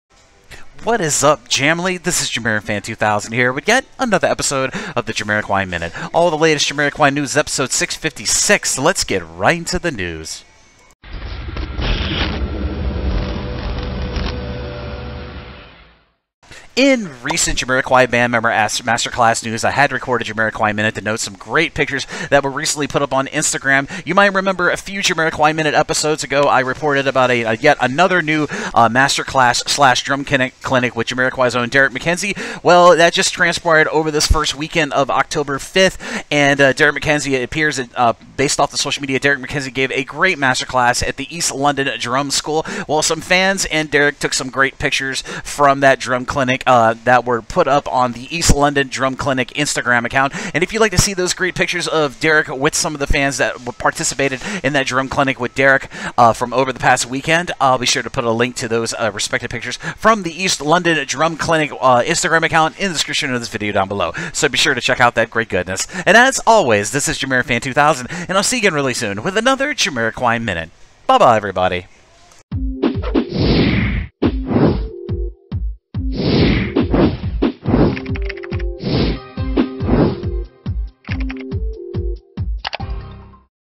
The Jamiroquai Minute With JamiroFan2000 | #656 | Fan Pictures From Oct. 2019 Derrick Masterclass!
Welcome to the NEW & IMPROVED "Jamiroquai Minute", Ep. #656, all the latest Jamiroquai News!

Here are links to everything discussed in today's episode:

"Derrick McKenzie Oct. 2019 Drum Masterclass | Picture #1" From East London Drum School Instagram:

"Derrick McKenzie Oct. 2019 Drum Masterclass | Picture #2" From East London Drum School Instagram: